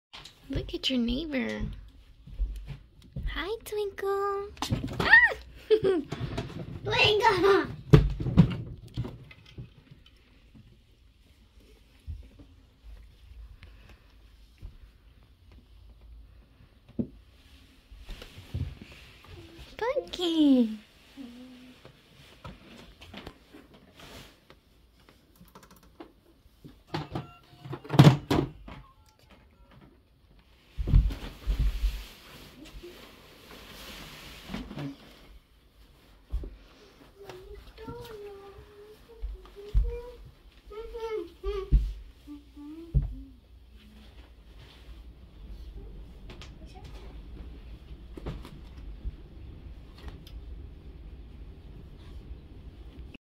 whoa, get back here
Shisha and Twinkle having fun in the little crib Pumpkin decided to stay back instead she wanted to show off Shisha's picture